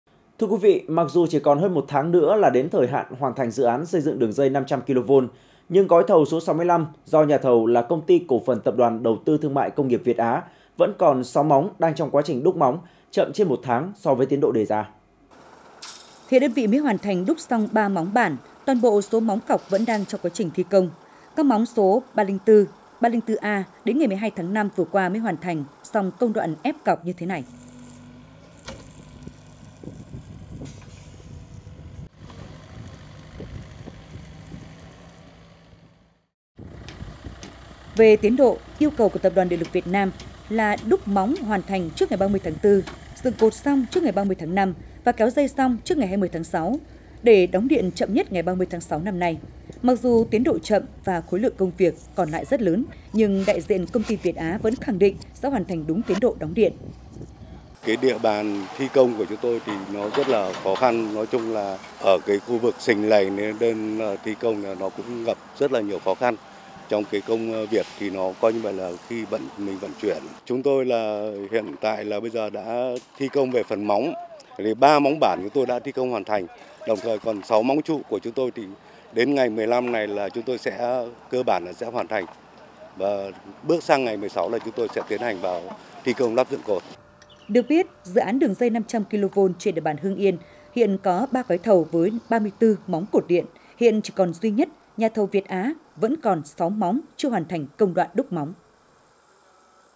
Đường dây 500 KV mạch 3 qua Hưng Yên chậm tiến độ
Hưng Yên TV | Mặc dù chỉ còn hơn 1 tháng nữa là đến thời hạn hoàn thành dự án xây dựng đường dây 500Kv nhưng gói thầu số 65 do nhà thầu là công ty Cổ phần Tập đoàn đầu tư thương mại công nghiệp Việt Á vẫn còn 6 móng đang trong quá trình đúc móng, chậm trên 1 tháng so với tiến độ đề ra.
Địa chỉ: 164 Nguyễn Văn Linh, thành phố Hưng Yên, tỉnh Hưng Yên
Tiktok: Truyền hình Hưng Yên
Facebook: Đài Phát thanh và Truyền hình Hưng Yên
Zalo: Đài PTTH Hưng Yên